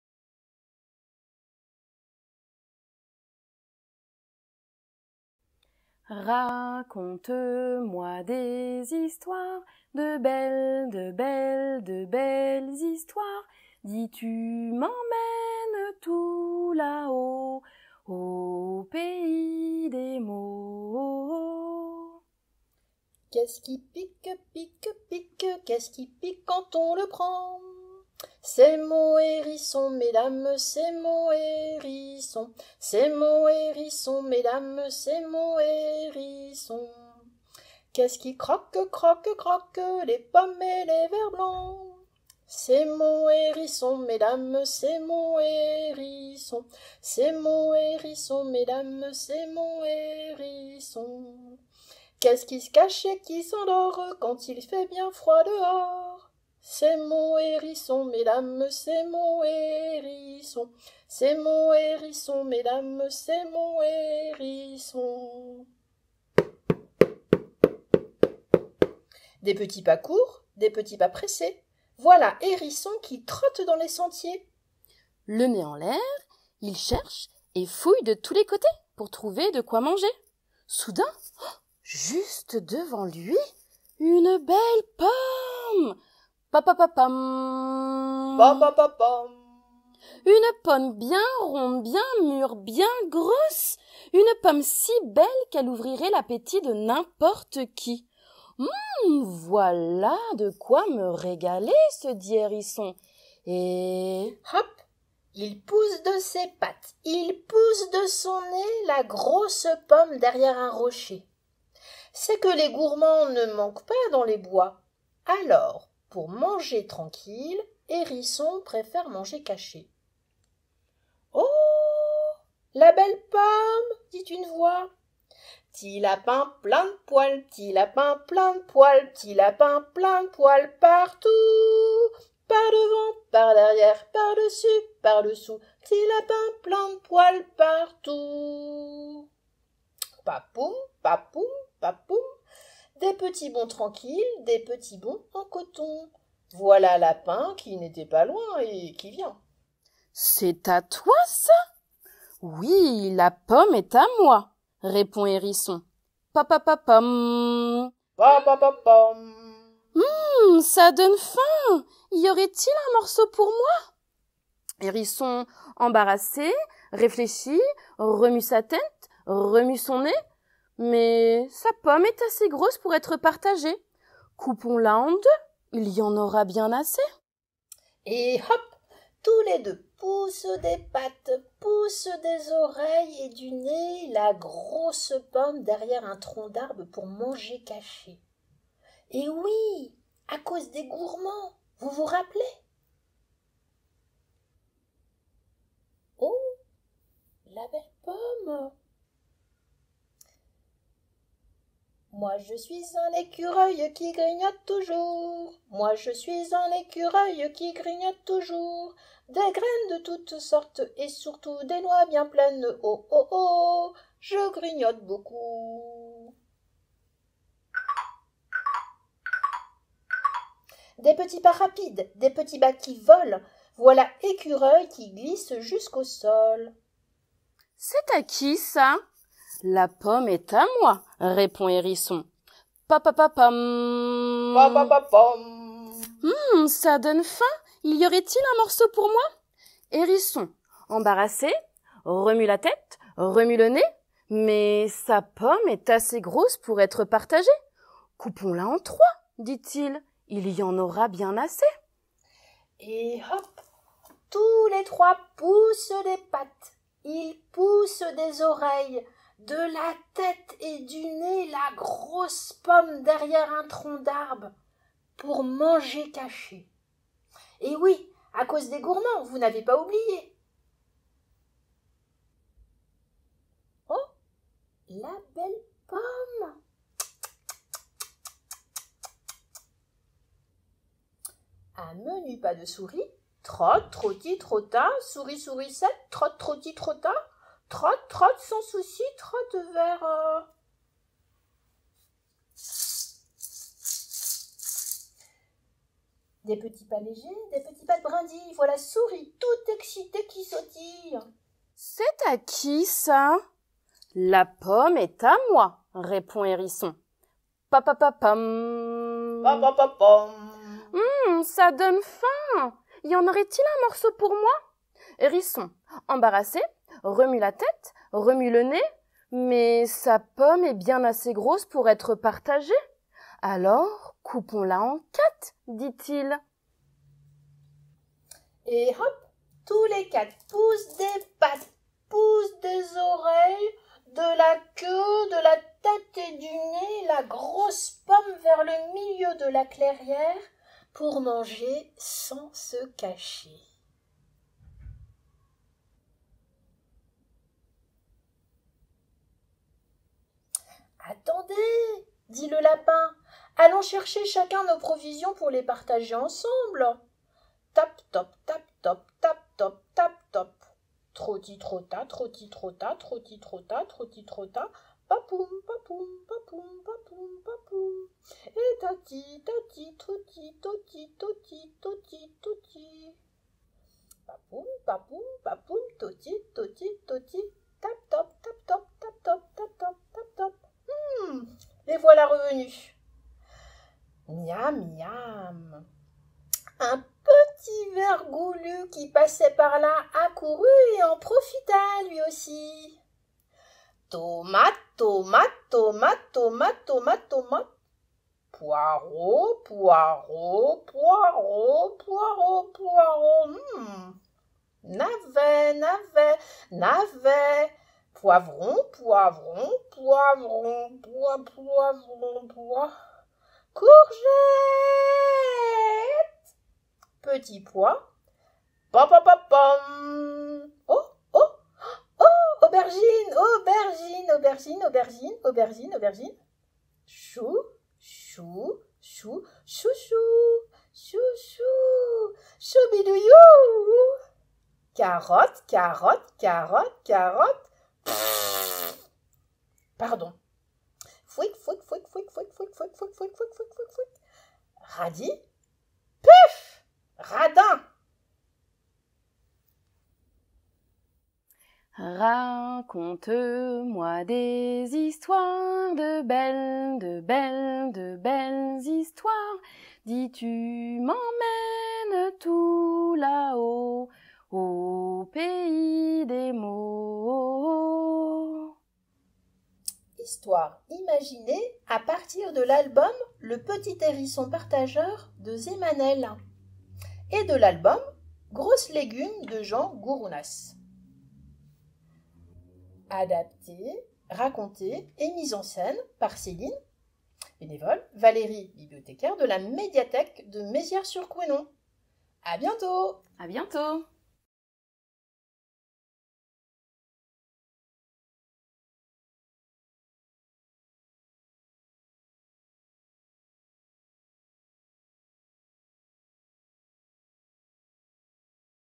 # Les p'tites farandoles d'histoires - Le hérisson partageur et ses amis
"Le hérisson partageur et ses amis"
Une histoire librement inspirée des albums pour enfants "Le petit hérisson partageur de Zemanel et de "Grosse légume" de Jean Gourounas.
Adaptée, racontée et mise en scène par Céline et Valérie de la médiathèque de Mézières sur Couesnon (Mars 2021)